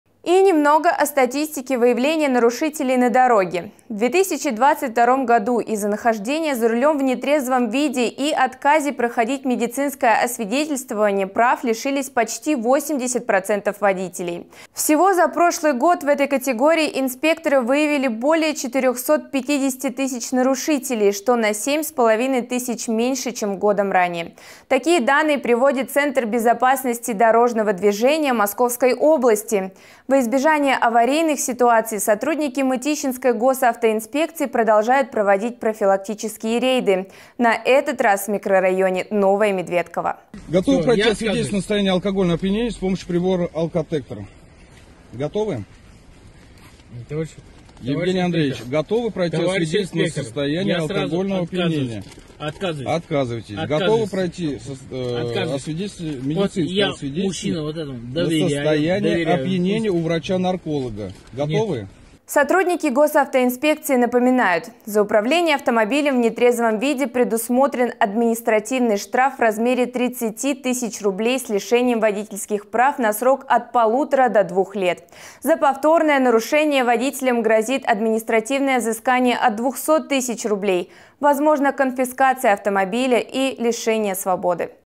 Сотрудники Госавтоинспекции продолжают выявлять нарушителей
В 2022 году из-за нахождения за рулем в нетрезвом виде и отказе проходить медицинское освидетельствование прав лишились почти 80% водителей. Всего за прошлый год в этой категории инспекторы выявили более 450 тысяч нарушителей, что на 7 с половиной тысяч меньше, чем годом ранее. Такие данные приводит Центр безопасности дорожного движения Московской области. Во избежание аварийных ситуаций сотрудники мытищинской Госавтоинспекции продолжают проводить профилактические рейды. На этот раз в микрорайоне «Новое Медведково». Сотрудники Госавтоинспекции напоминают, за управление автомобилем в нетрезвом виде предусмотрен административный штраф в размере 30 тысяч рублей с лишением водительских прав на срок от полутора до двух лет. За повторное нарушение водителям грозит административное взыскание от 200 тысяч рублей, возможна конфискация автомобиля и лишение свободы. (12+)